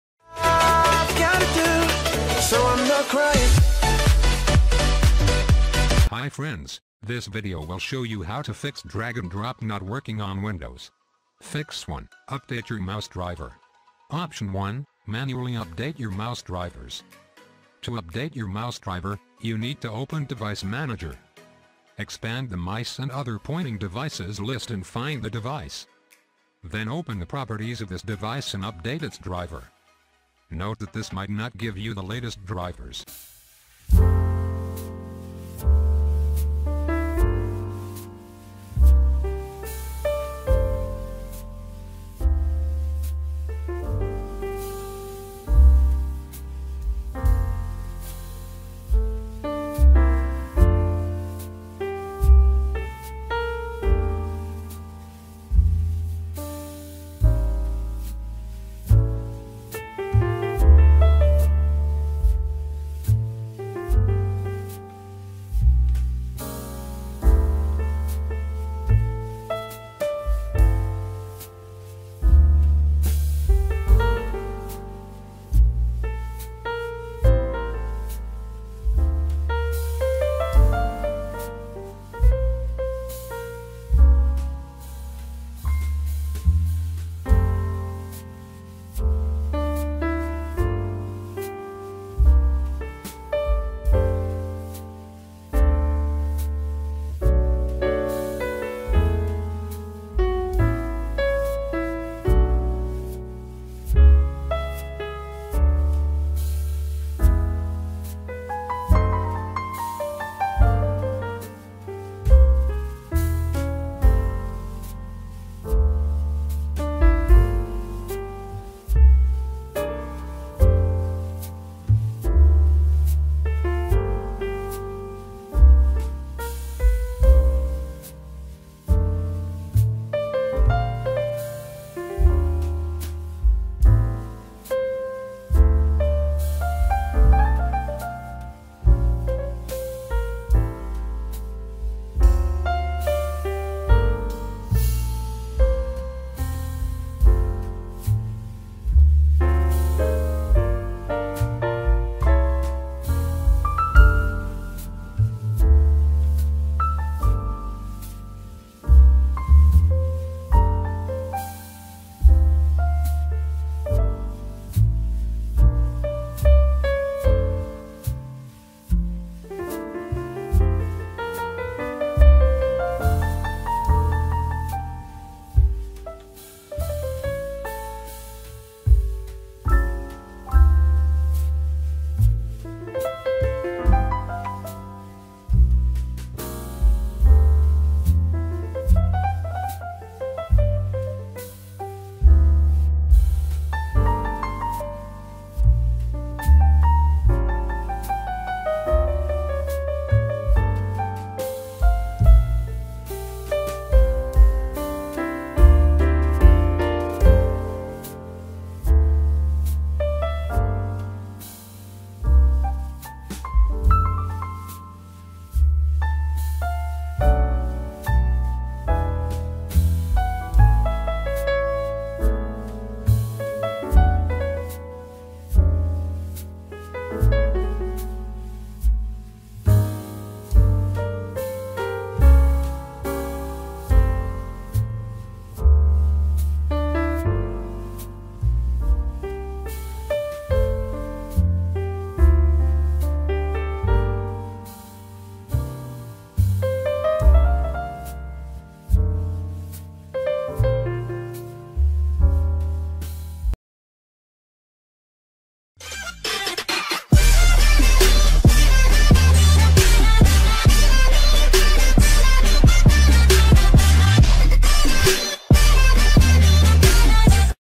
How to Fix Drag and Drop Not Working on Windows 10 - laptop drag and drop not working2020
If you can’t drag and drop in Windows 10/8/7, this video is helpful for you.
Try these fixes:
1. Update your mouse driver
2. Restart Windows Explorer
3. Run the system file checker: sfc /scannow
4. Use DISM command tool: DISM.exe /Online /Cleanup-image /Restorehealth

How to fix drag and drop in Excel not working:
How To Fix FL Studio drag and drop not working: